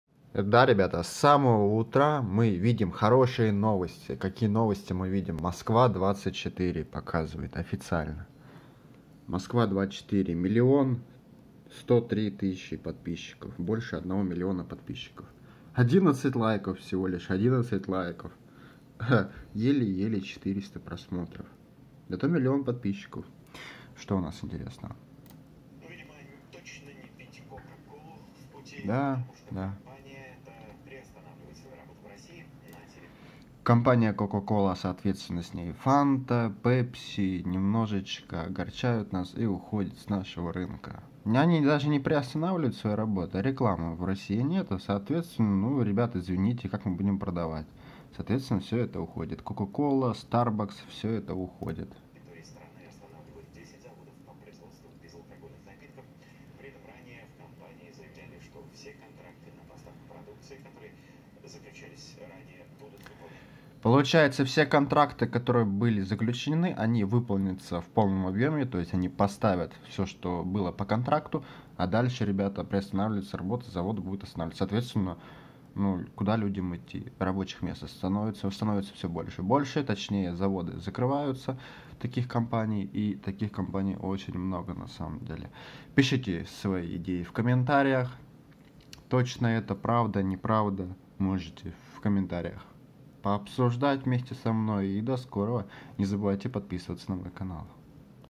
Coca-Cola приостанавливает всю деятельность в России\Москва 24\Что делать\Чем заменят\
Coca-Cola приостанавливает всю деятельность в России, говорится в заявлении на сайте компании. «Мы будем продолжать отслеживать и оценивать ситуацию по мере развития обстоятельств», — отметили в сообщении.
Нету рекламы мы уходим с рынка в Росиий\
Что нам покажет Москва 24?